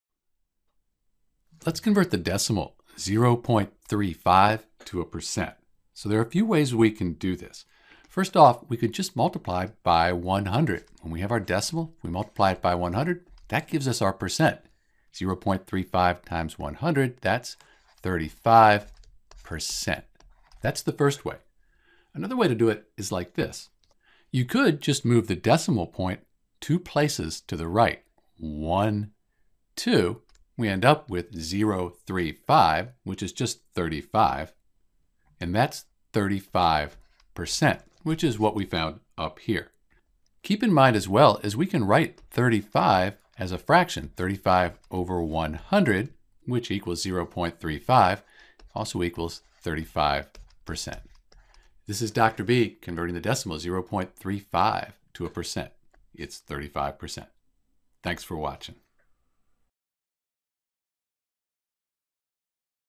0.35 as a Percent
When we convert a decimal, like 0.35 to a percent we can quickly find the answer by doing the following:

Decimal x 100 = ______ %

0.35 x 100 = 35%

So we multiply 0.35 by 100 to convert 0.35 to a percent. When we do that, we find that 0.35 is 35%.

Note that the decimal 0.35 is less than one so we should have a percentage less than 100%. This is a helpful way to check your work.

--- Steps to Change 0.35 to a Percentage—

1. Multiply the decimal by 100.
2. Check your work to see if your answer is reasonable.
Note you can also change the decimal 0.35 to a percent by moving the decimal place two places to the right. This will also give you 35%